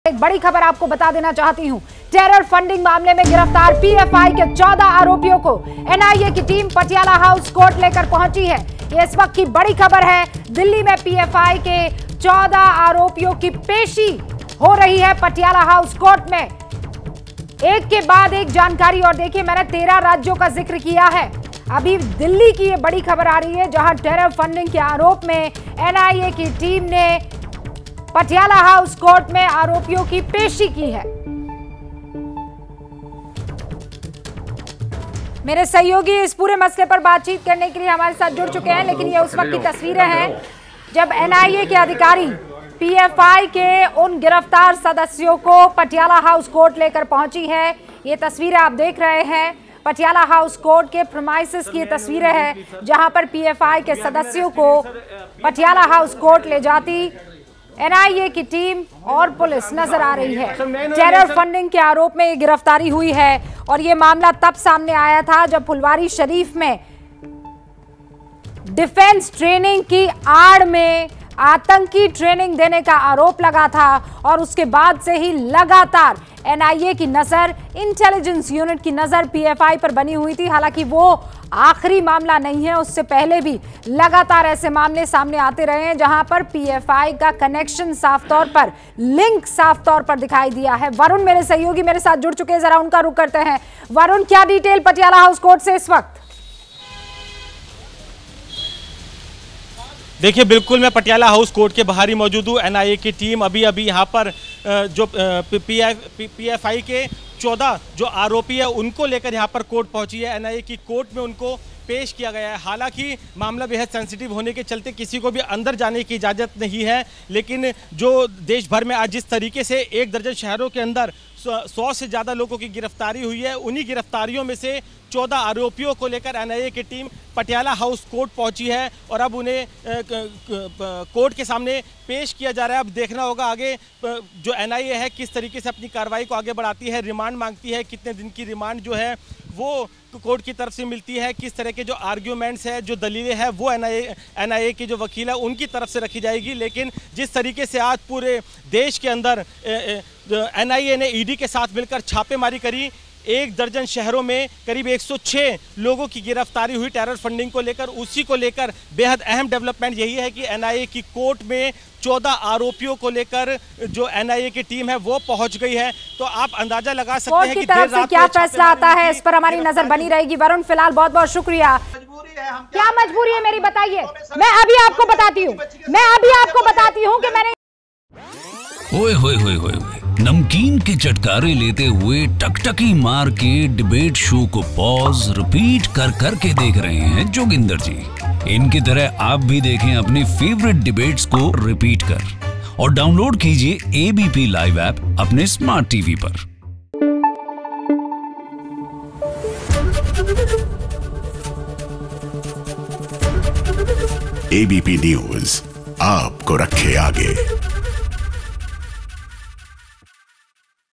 Delhi में PFI के 14 आरोपियों की पेशी, टेरर फंडिंग के आरोप में हुई है गिरफ्तारी | Breaking News
News: Delhi में PFI के 14 आरोपियों की पेशी, टेरर फंडिंग के आरोप में हुई है गिरफ्तारी | Breaking News